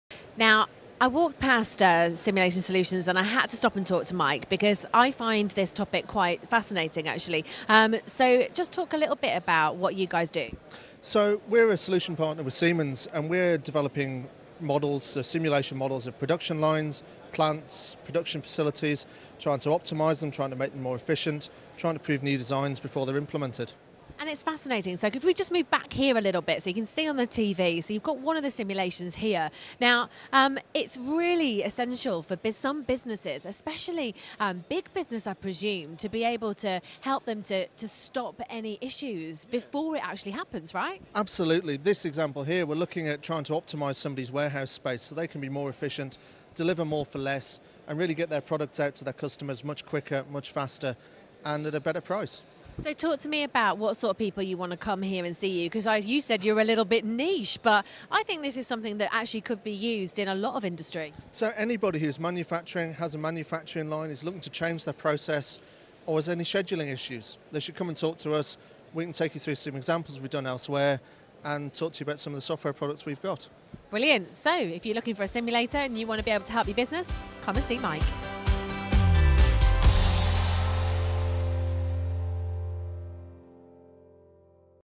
Mike Smith from Simulation Solutions speaking about simulation at the Packaging Innovations show
It's been a great couple of days at the PackagingInnovation show, our Michael Smith has been in demand on the stand talking about the work we do with manufacturers to review production processes and cut operating costs. Whether it's planning for a new factory, introducing new machinery, optimising an existing site or more, simulations made using Siemens Plant Simulation software to create virtual data to inform decision making and help you progress with certainty.

To find out more and arrange a chat with Mike and the team at Simulation Solutions, get in touch through our